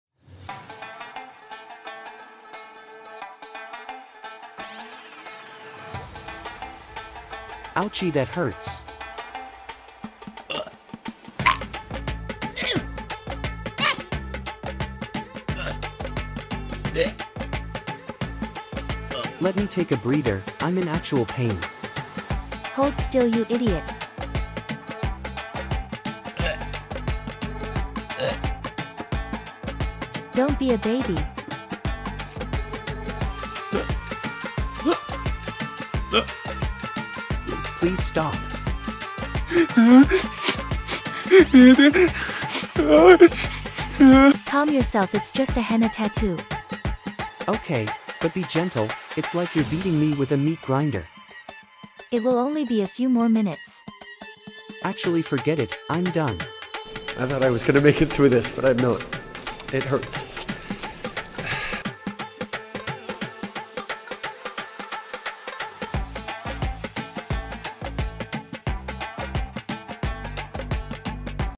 Justin Trudeau Getting Ready For His First Official Visit To India | Henna Tattoo | Political Satire
Why is Justin Trudeau such a clown you ask? Well that's hard to say, but what if during his first official visit to India back in the day, Justino decided to get a Henna Tattoo in order to Patronize the good people of India. You know, as is his way, he always likes to acclimatize to other people's cultures, you've seen the pictures. In this nonsensical and satirical masterpiece of fun and goofball, we look at what might have happened during Justin's first misadventure to the world's largest democracy. Won't you join us?